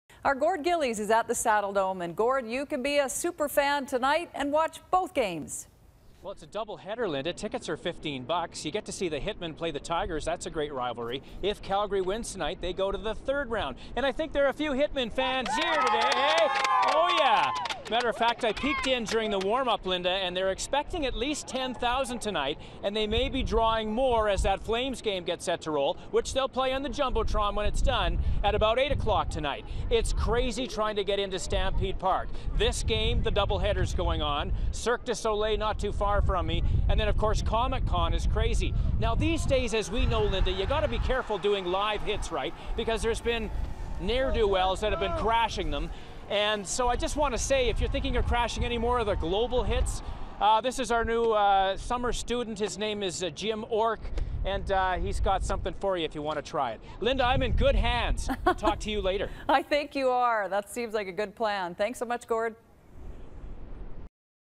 Hockey action at the Saddledome
Gord Gillies explains how fans can watch the away Flames games at the Saddledome and previews game 5 of the Hitmen vs Tigers

For the latest from Gord Gillies follow him on Twitter @Gord_Gillies